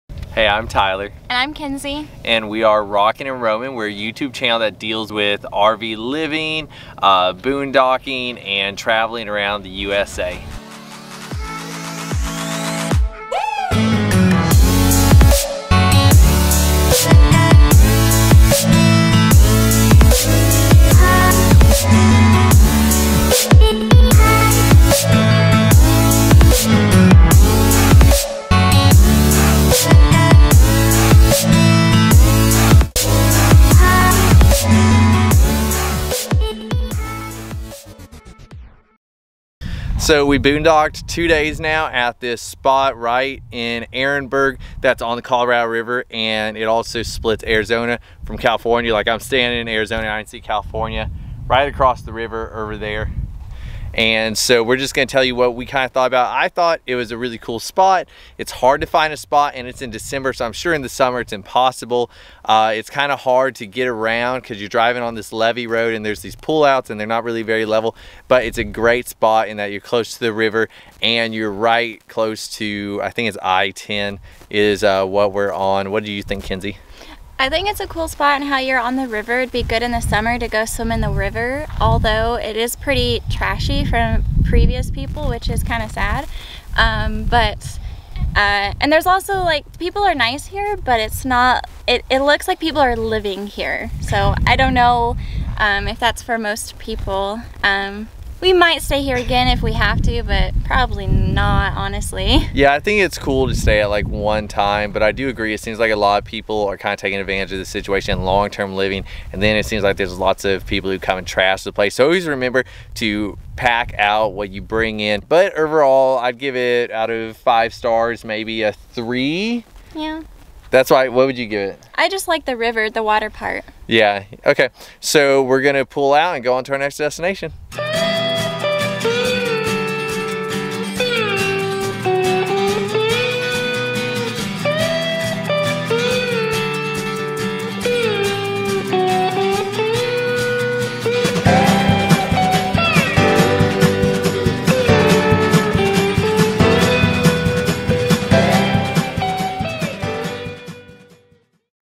Free Camping in Ehrenberg Arizona on the Colorado River.
This is our review of the boondocking areas on the Colorado River in Ehrenberg, Arizona. Let us know how your experience was so that others watching can decide if they want to stay here.